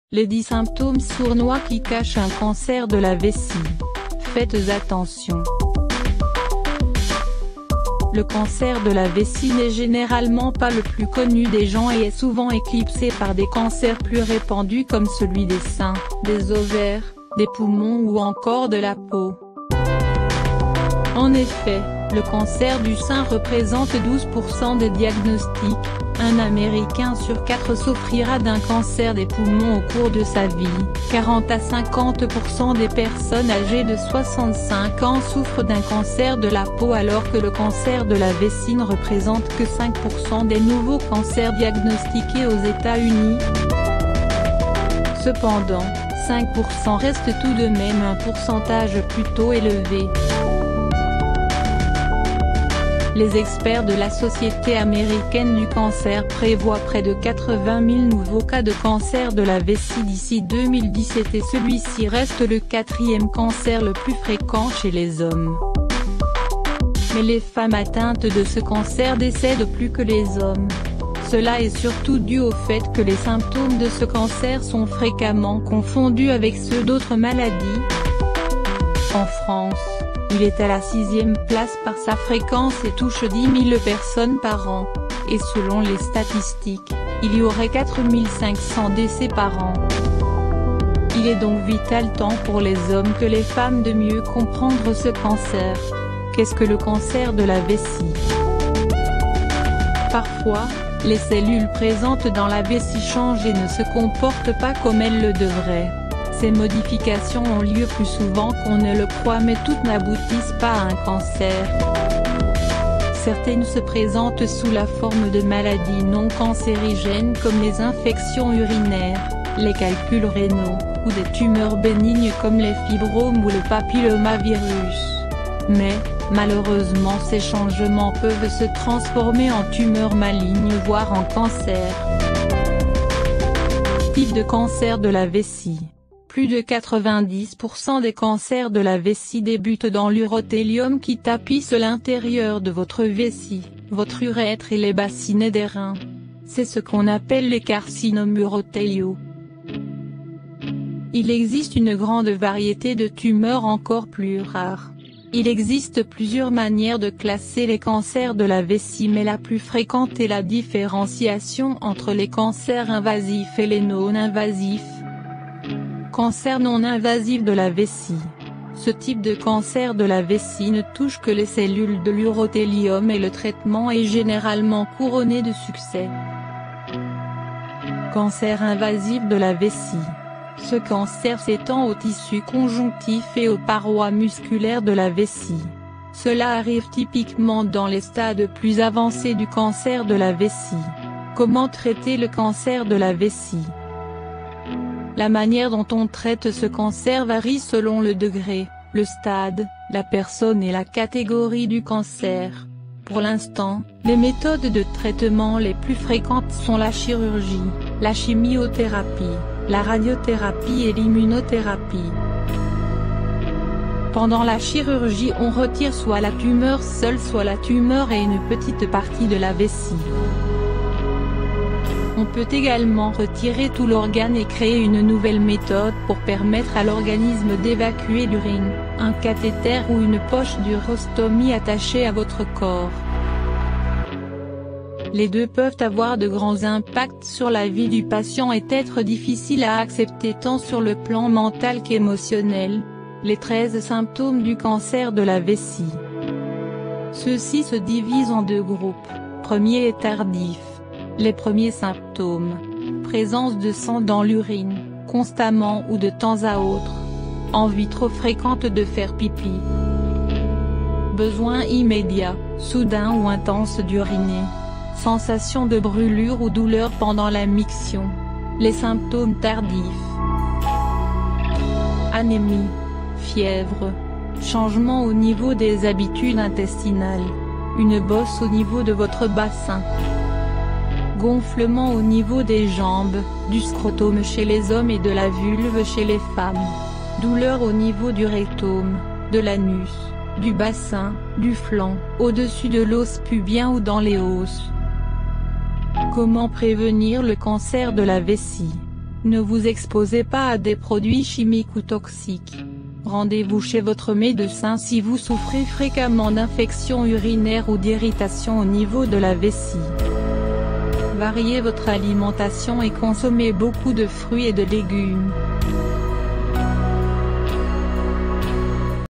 Les 10 symptômes sournois qui cachent un cancer de la vessie. Faites attention !
Le cancer de la vessie n’est généralement pas le plus connu des gens et est souvent éclipsé par des cancers plus répandus comme celui des seins, des ovaires, des poumons ou encore de la peau.